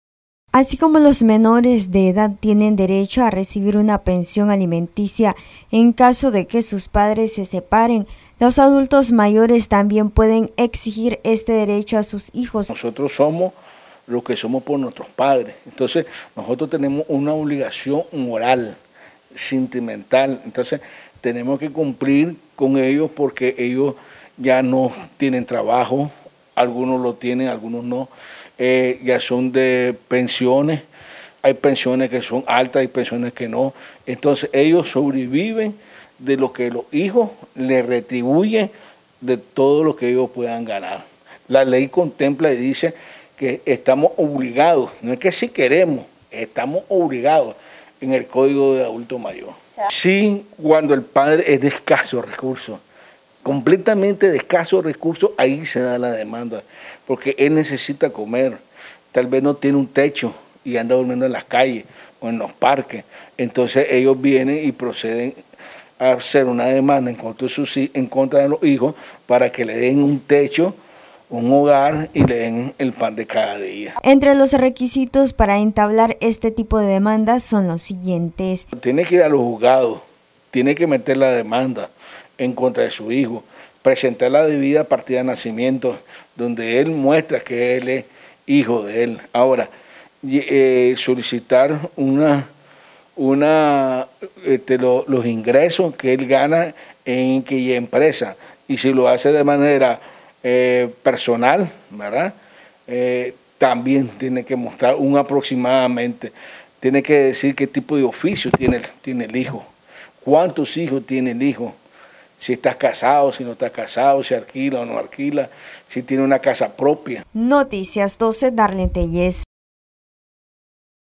Adultos mayores pueden pedir pensión a sus hijos
Noticias12Nicaragua Abogado Vladimir Valle afirma que en Nicaragua un 30% de los adultos mayores solicitan pensión alimenticia a sus hijos, así mismo dio a conocer como ejecutarse.